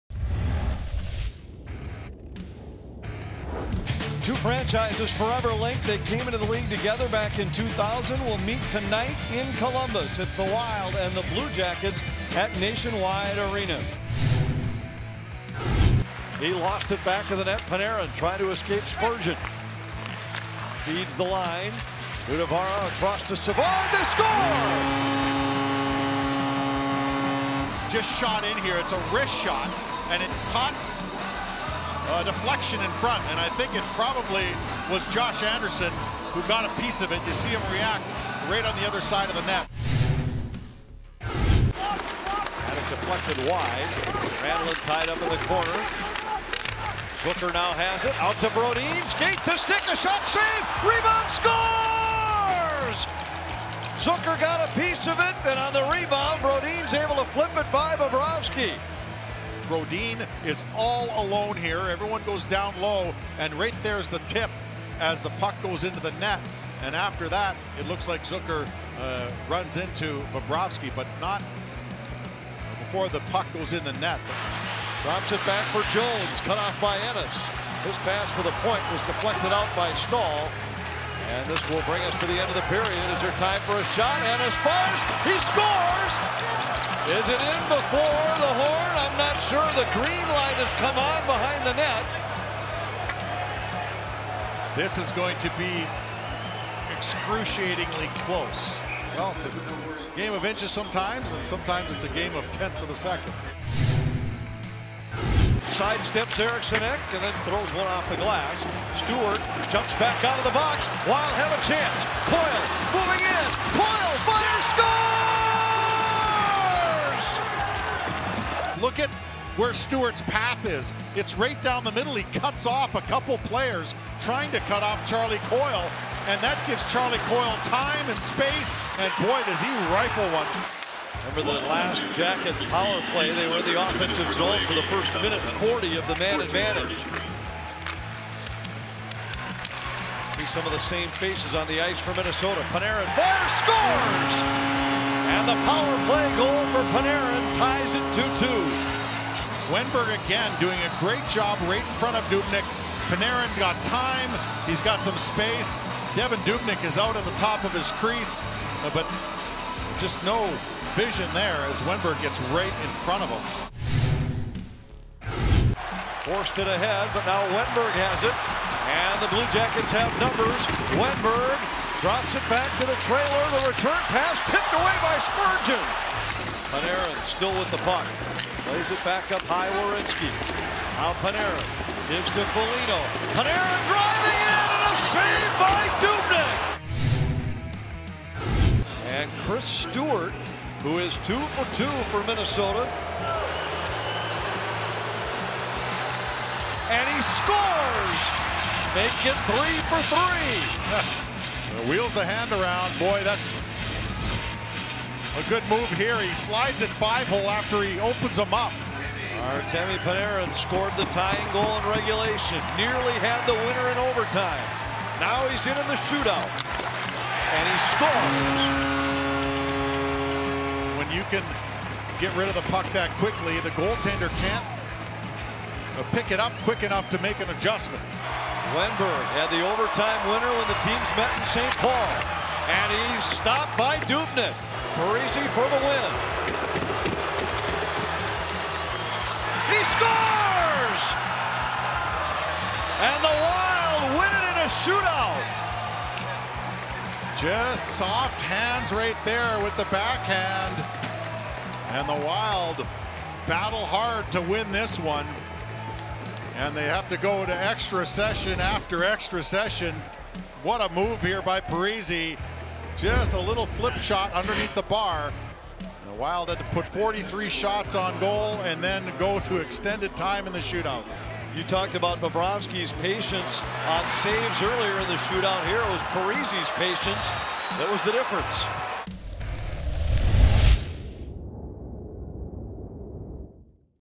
Minnesota Wild vs Columbus Blue Jackets (3-2 Penalties) – Jan. 29, 2018 | Game Highlights | NHL 2018
Today I'm here to show all goals and highlights between those to teams when they meet 29/1-18.